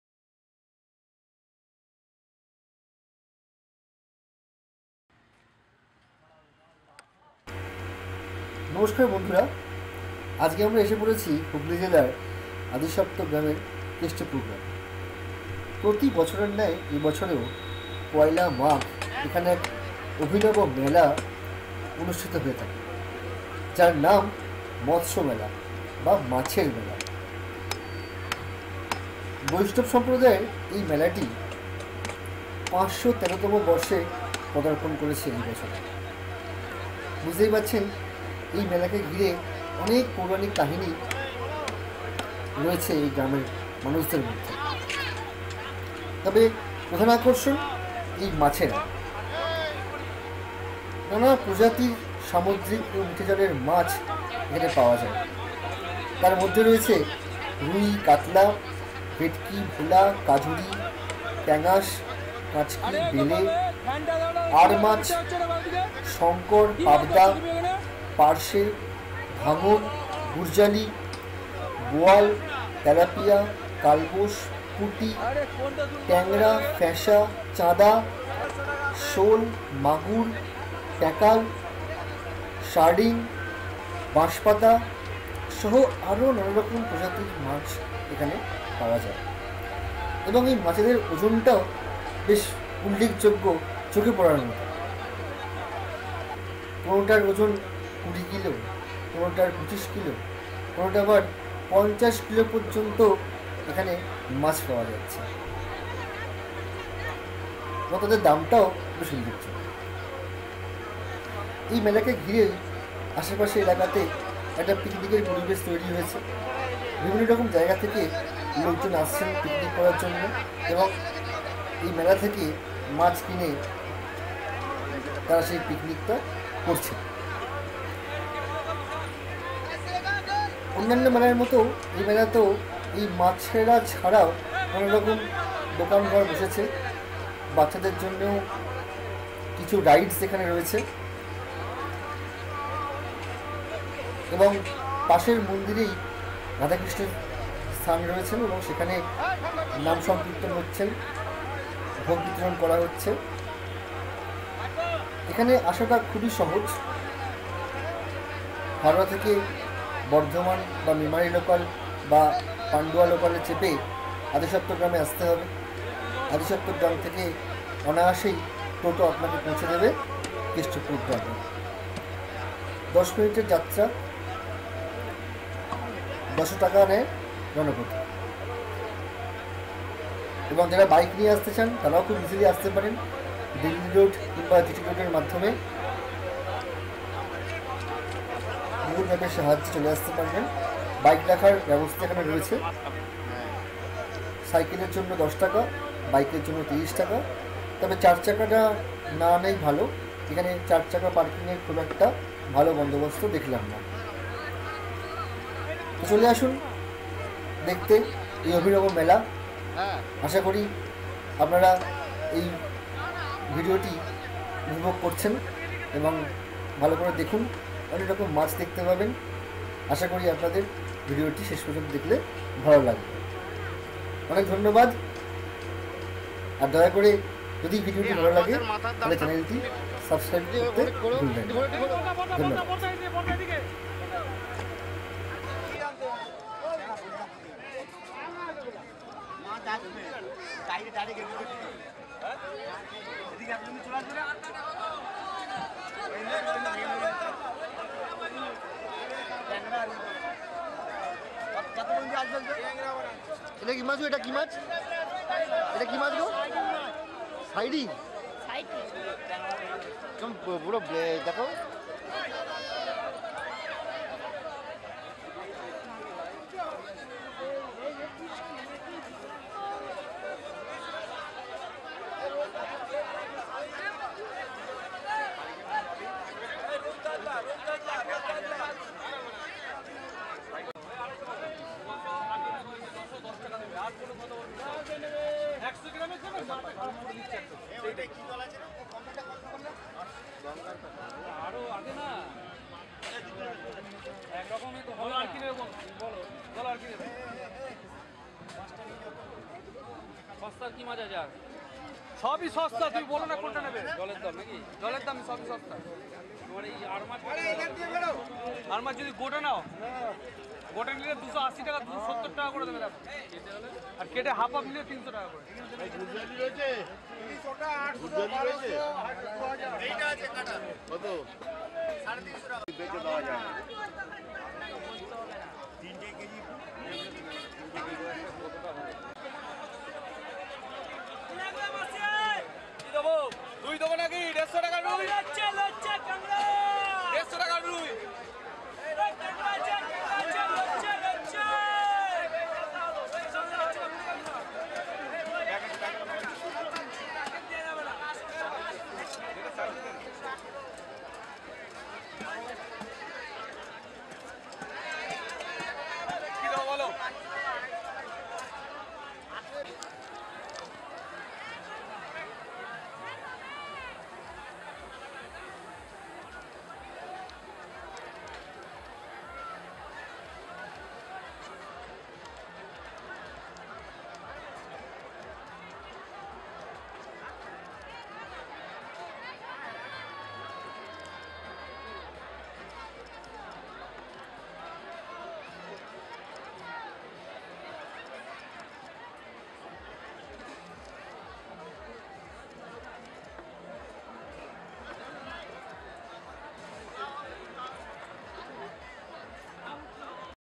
Motsyo Mela| Fish Fair, Adi Saptagram, Hooghly, 16th January 2020
হুগলী জেলার আদিসপ্তগ্রাম এর কেষ্টপূর। এই বছর ৫১৩ তম মৎস্য মেলা অনুষ্ঠিত হচ্ছে। শুধু মাত্র একদিনের জন্যে বসে মাছের মেলা। ১লা মাঘ।
খুব ই সহজ ,হাওড়া -বর্ধমান মেন্ লাইনের অন্যতম স্টেশন আদিসপ্তগ্রাম I এখানে নেমে স্টেশন থেকেই পাবেন টোটো,অটোI তারাই আপনাকে নিয়ে যাবে কেষ্টপুরের মাছের মেলাযI যাঁরা দুচাকা বা গাড়ি তে যেতে চান তাদের জন্য তো দিল্লী রোড আর GT রোড এর মাধ্যমে খুব সহজেই চলে আসা যাবে এখানে।